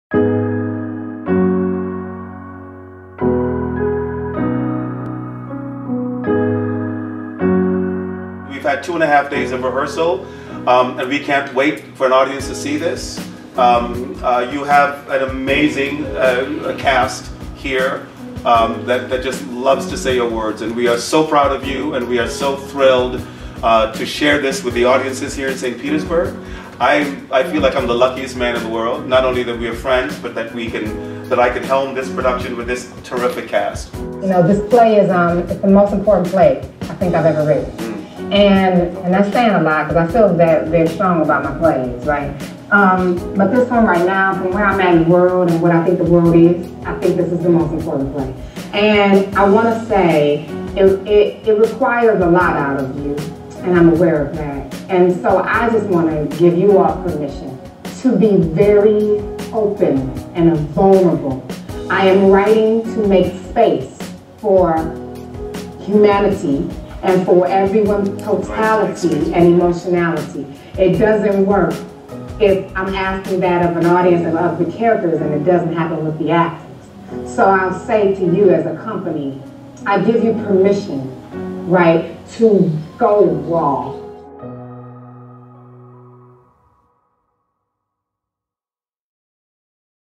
American Stage: Message from PIPELINE Playwright Dominique Morisseau
The cast of PIPELINE had the unique opportunity to discuss PIPELINE with playwright and MacArthur Foundation 'Genius Grant' Recipient Dominique Morisseau.